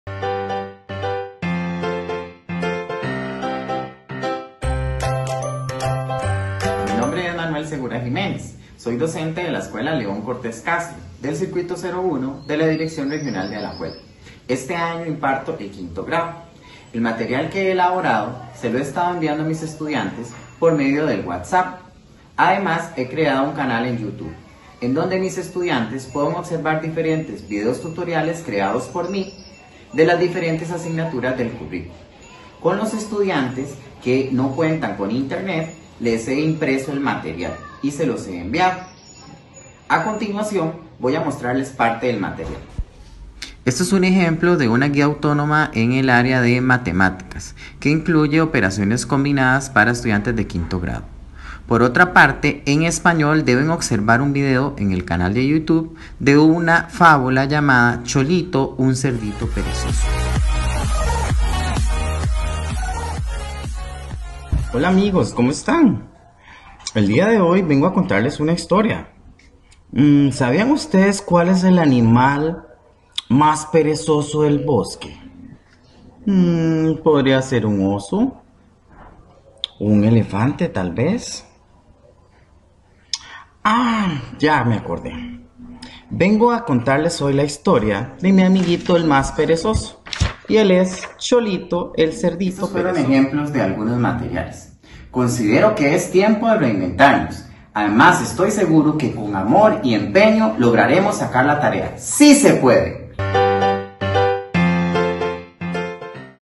MANUEL SEGURA JIMÉNEZ LA EDUCACIÓN CONTINÚA EN CASA
COVID 19: La Educación Continúa en Casa🏠
Cambia la estrategia…No la meta 🚩

Manuel Segura, Escuela León Cortés Castro.

Con su material digital y su canal en Youtube... el futuro no se detiene 💪🏼